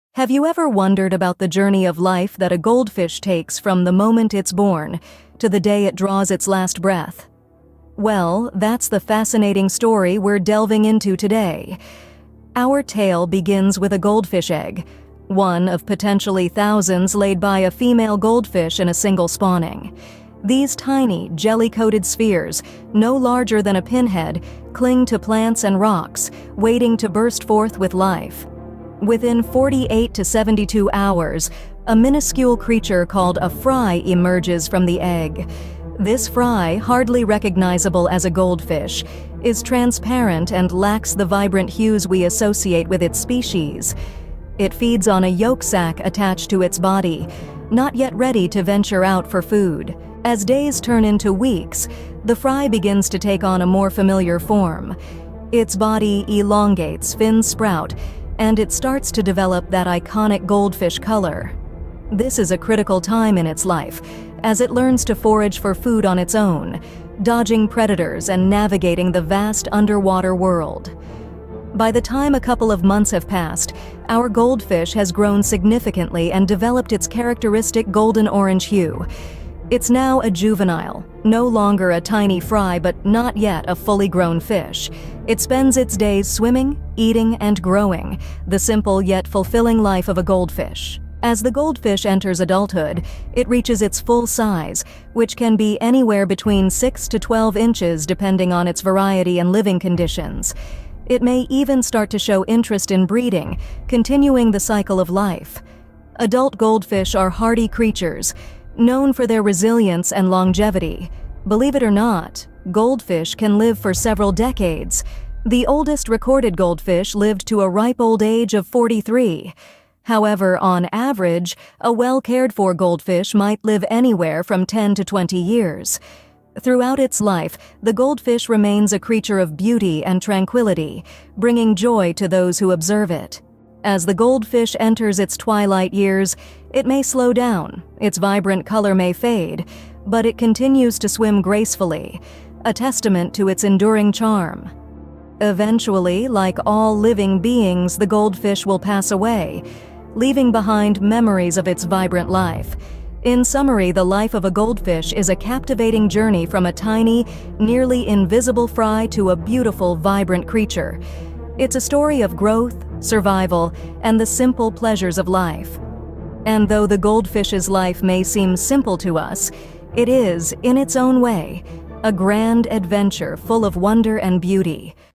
Lifecycle of a Goldfish: From Egg to Elder
This illustration charts the lifecycle of a goldfish, showcasing each stage from egg to adulthood. It begins with eggs hatching into fry, which then grow into juveniles exhibiting golden hues. As adults, goldfish reach full size and color vibrancy, and the cycle concludes with elder goldfish showing signs of aging.